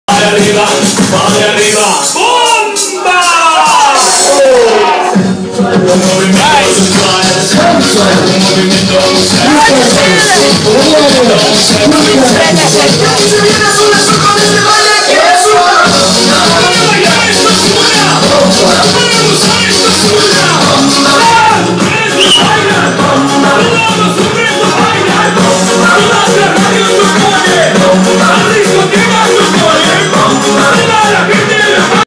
tardona sexi
ceci balla con bimbi di 16 anni